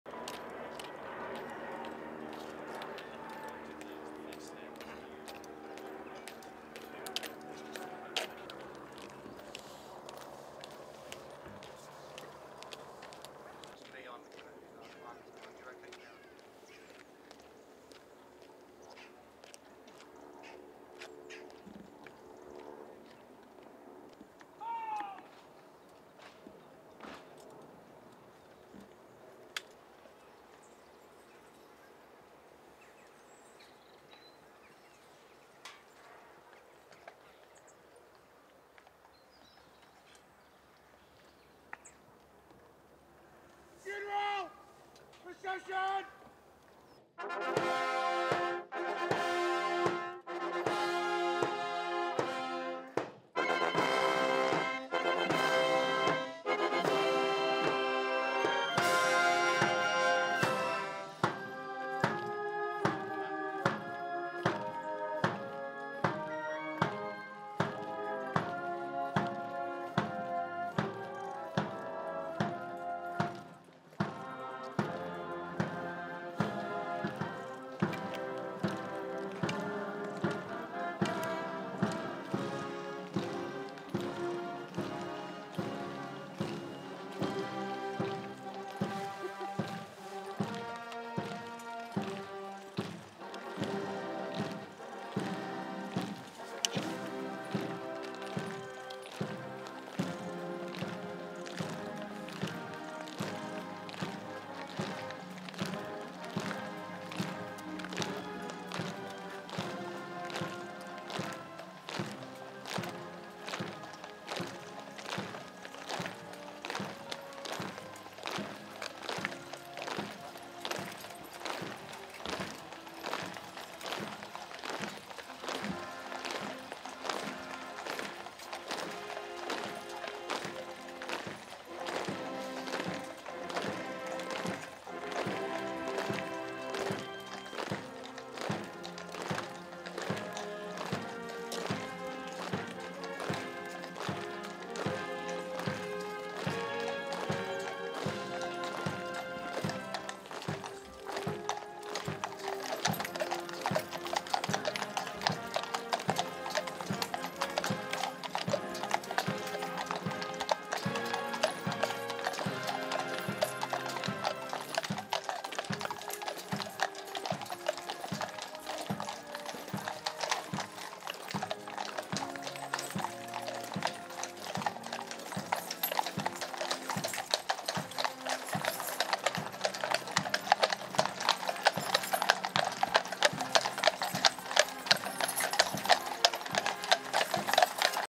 UK Queen Elizabeth II Windsor Arrival Hearse
(19 Sep 2022) Arrival of State Hearse at Windsor